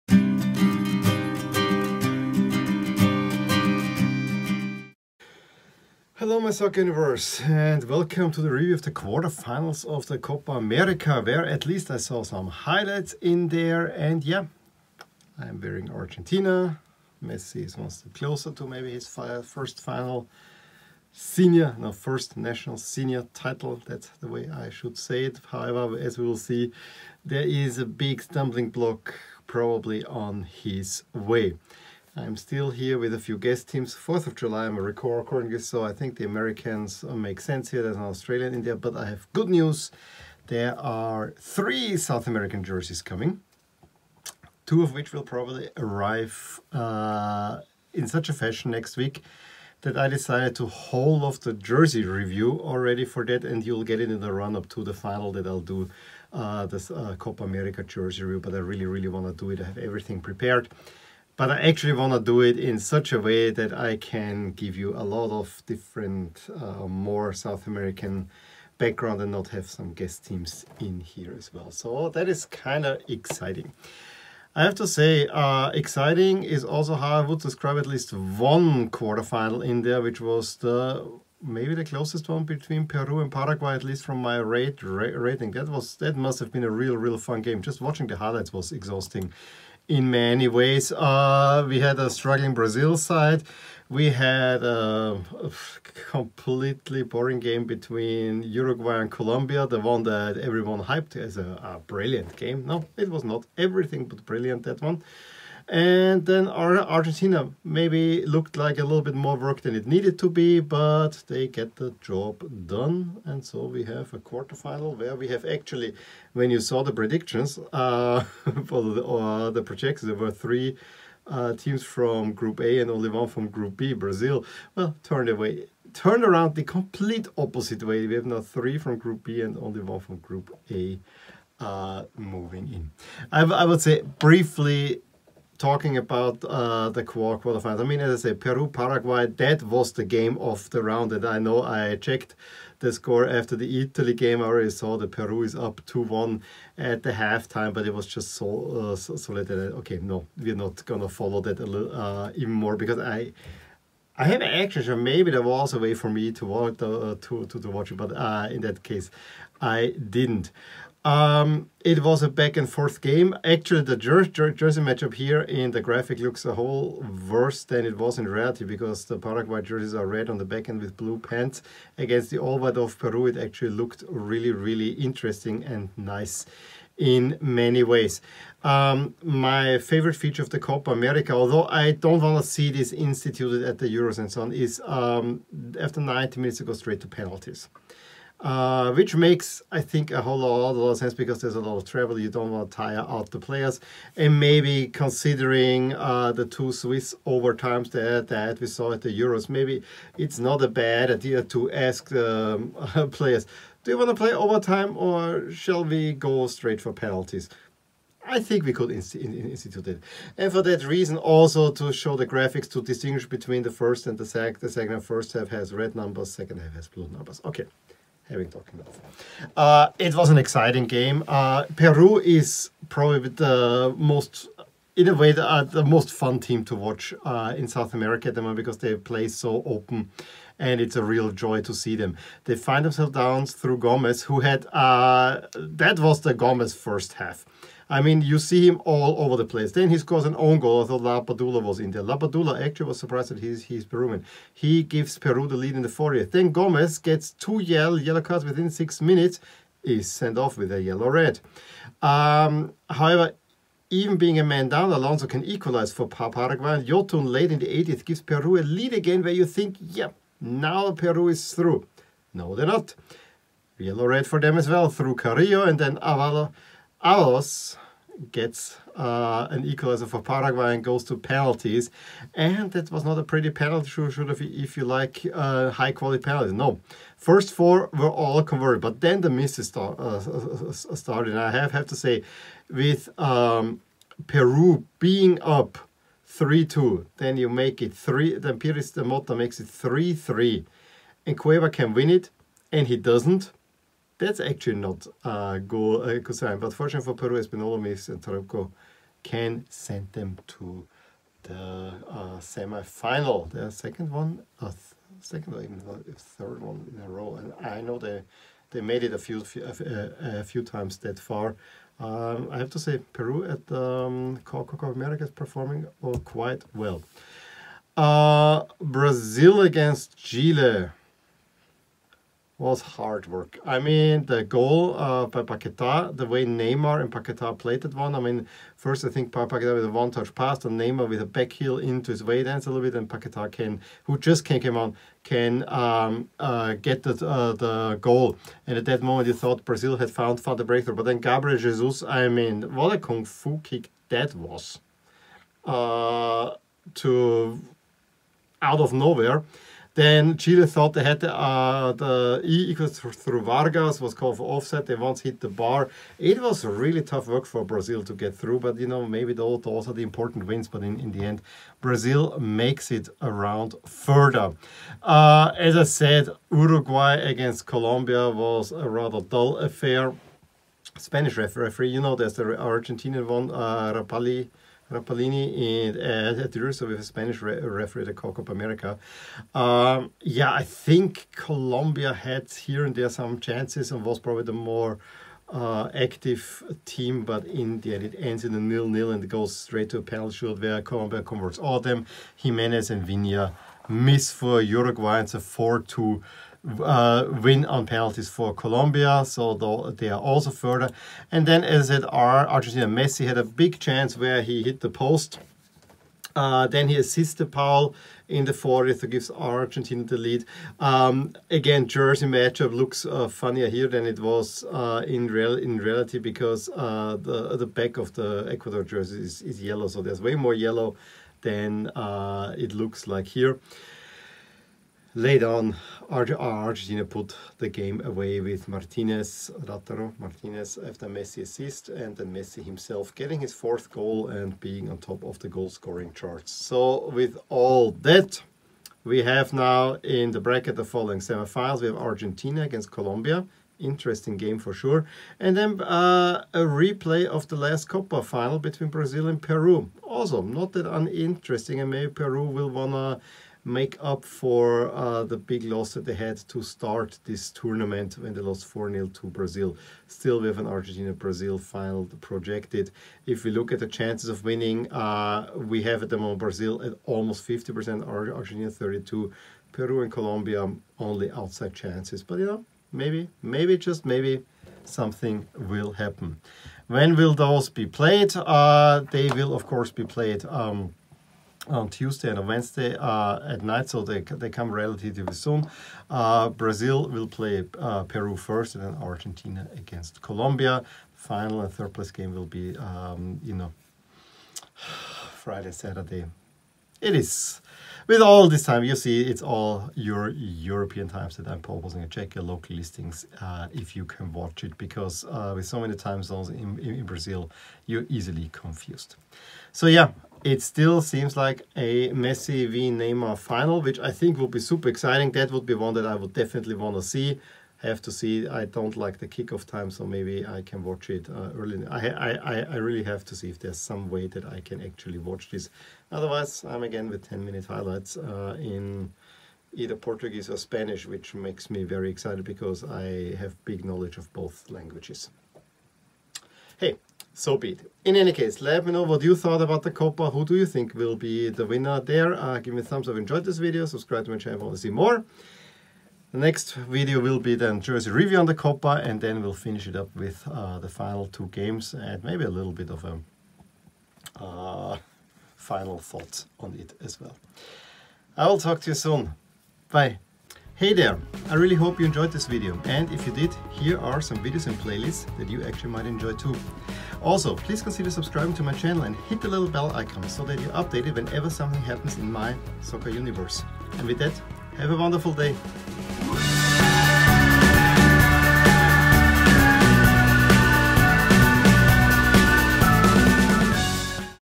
Brazil Struggling, Argentina Cruising and Penalty Shootouts
Not all the quarter final action was great, but Peru and Paraguay played out an epic one culminating in a thrilling penalty shootout with as many twists and turns as the game itself. Brazil really had to dig deep - especially after Gabriel Jesus was sent off, while Colombia oust Uruguay on penalties. And then, Leo Messi leads Argentina into the semifinals with two assists and a goal. So, we are still set up for the big Messi-Neymar final!